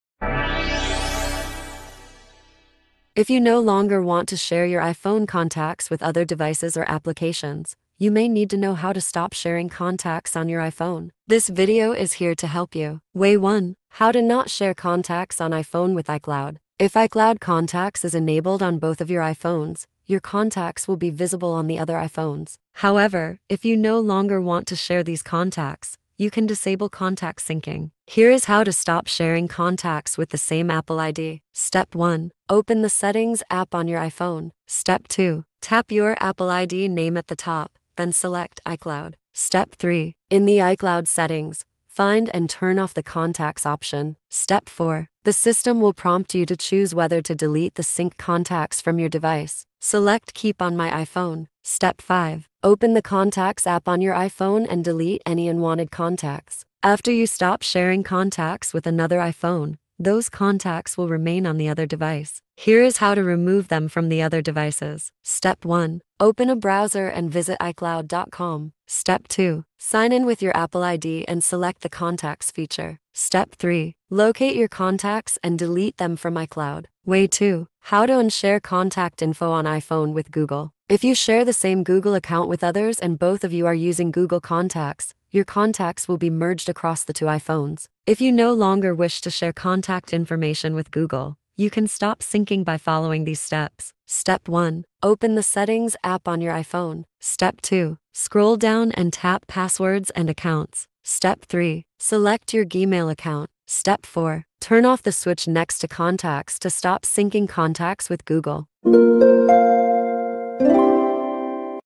How to Stop Sharing Contacts on iPhone? [2 Ways]
Are you looking for how to stop sharing contacts on your iPhone? You are in the right place. Come in and learn the detailed steps in this guide.

00:00 Start
00:13 Way 1. How to Not Share Contacts on iPhone with iCloud
01:16 Way 2. How to Unshare Contact Info on iPhone with Google